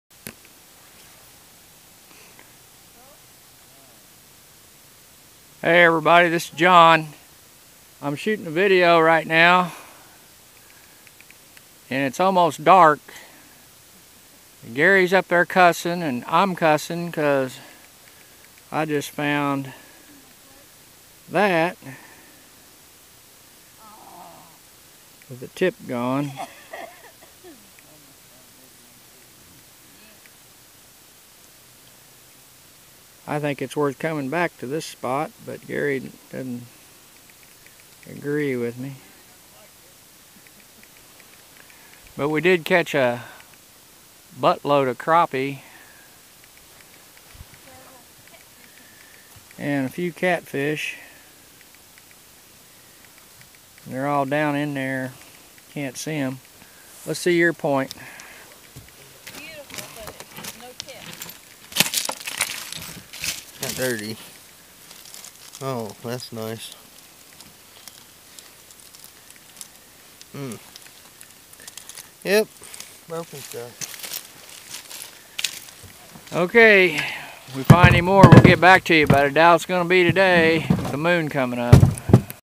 Moon light bay arrowhead hunting!!
Found a couple of brokes just before dark !! lots of catfish and crappie too!!